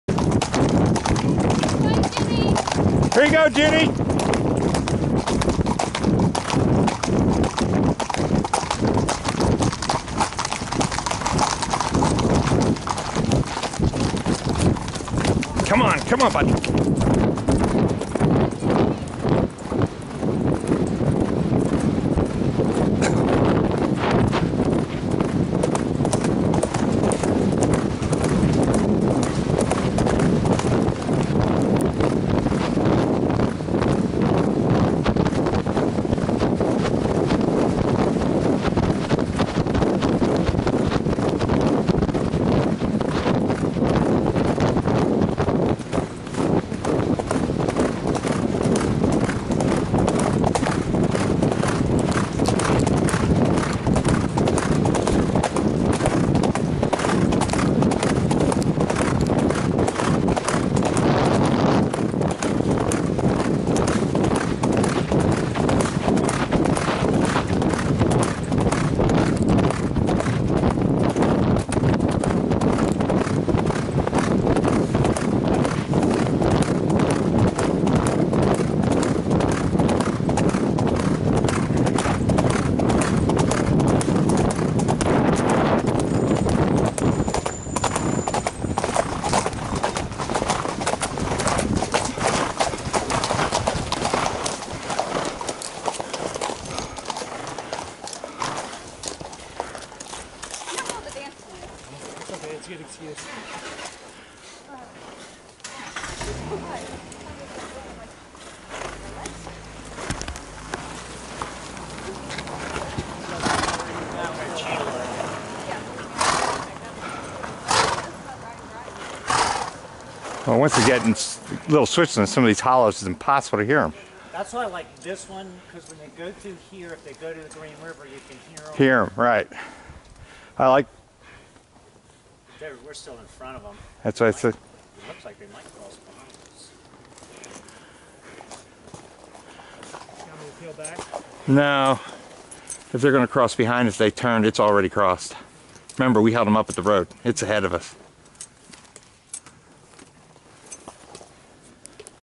Video 4 March 5th
$ of 5 videos of a fast moving hunt, all videos are shot at most 2 minutes apart.